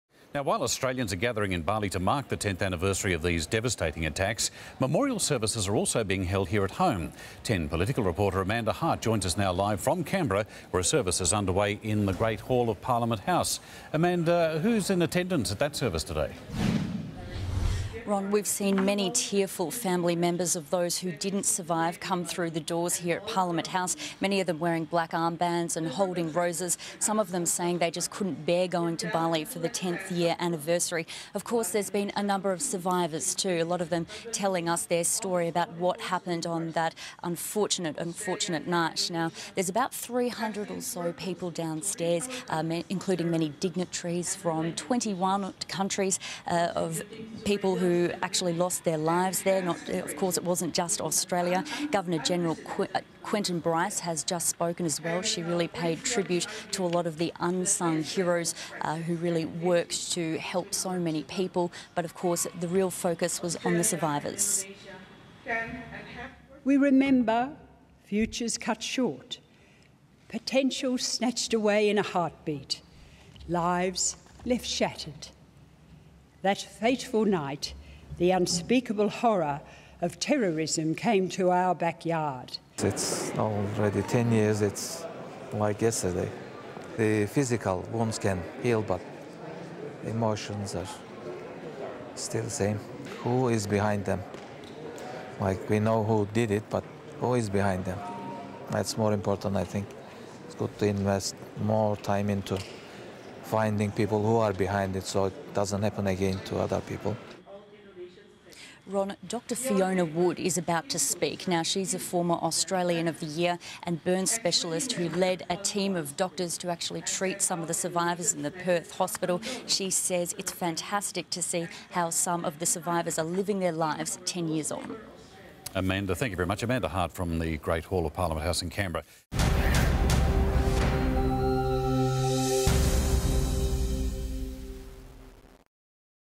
Australia honours Bali victims
Australians are gathering in Bali to mark the 10th anniversary of the devastating Bali attacks, with memorial services being held here at home